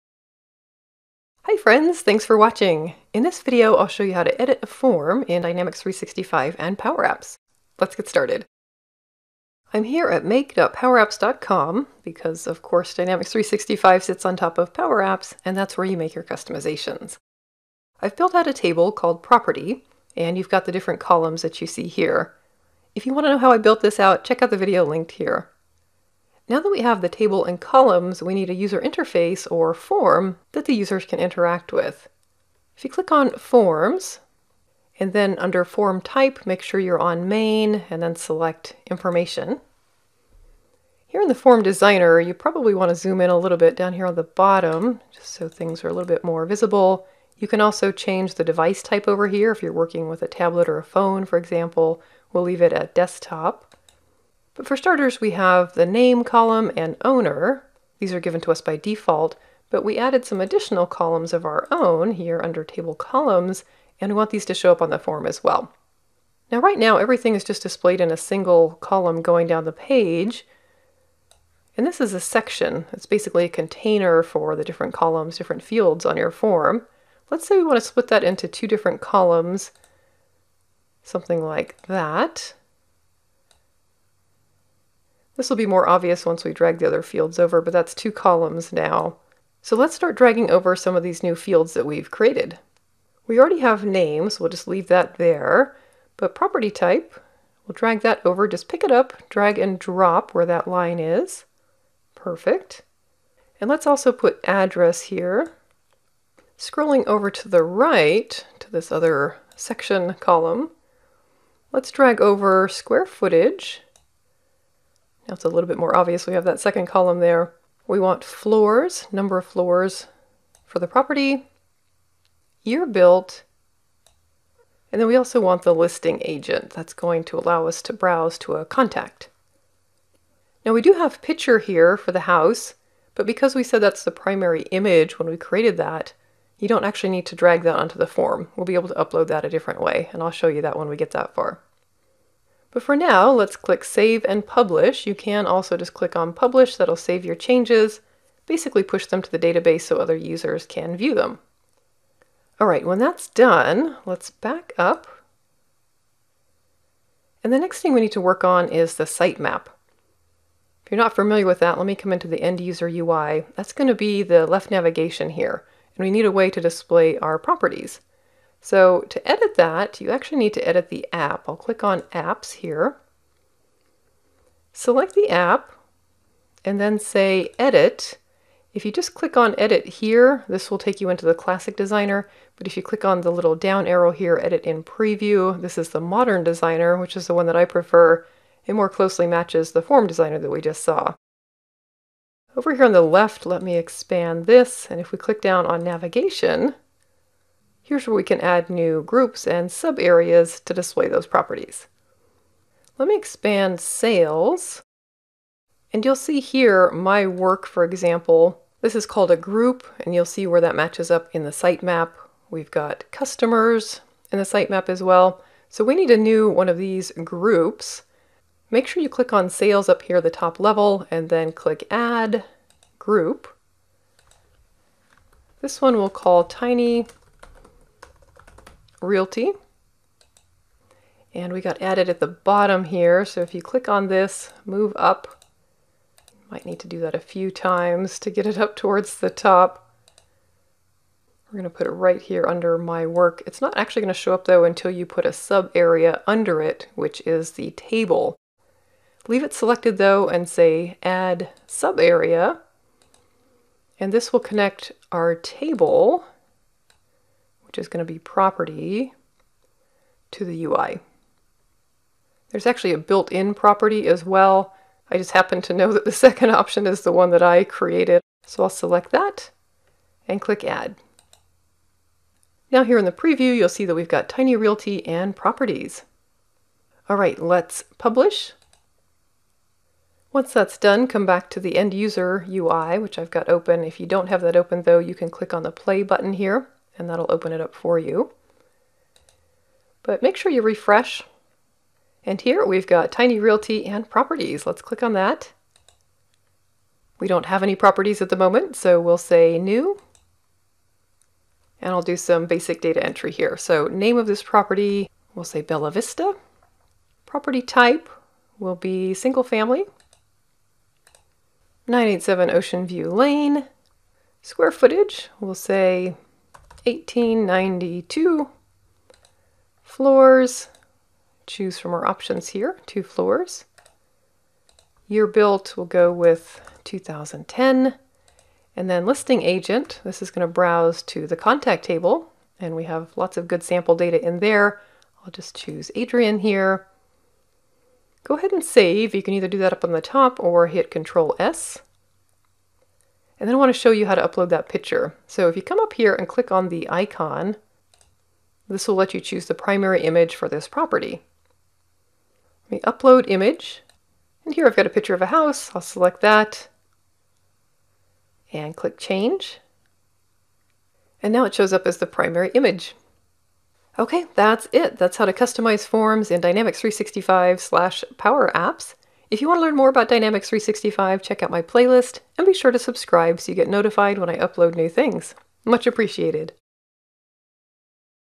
Edit a Form in Dynamics 365/Power Apps for the Sales App | Dynamics 365 for Beginners
In Dynamics 365, the form is the main UI that users interact with to view and enter data in a table.  It’s easy to customize in the Power Apps modern form and then pull it into the Sales app through the sitemap.

In this hands-on tutorial, I’ll show you how to add columns/fields to a form in Power Apps, how to edit the Dynamics 365 Sales app and customize its sitemap, and finally, how to enter data in the new form.

00:00 – Video intro
00:10 – Editing the Main form for a table in Power Apps
02:36 – Editing the Dynamics 365 Sales app
03:11 – Editing the sitemap for the Dynamics 365 Sales app
04:55 – Entering data in the Dynamics 365 Sales app

If you want to set up a free 30-day trial of Dynamics 365, check out another one of my videos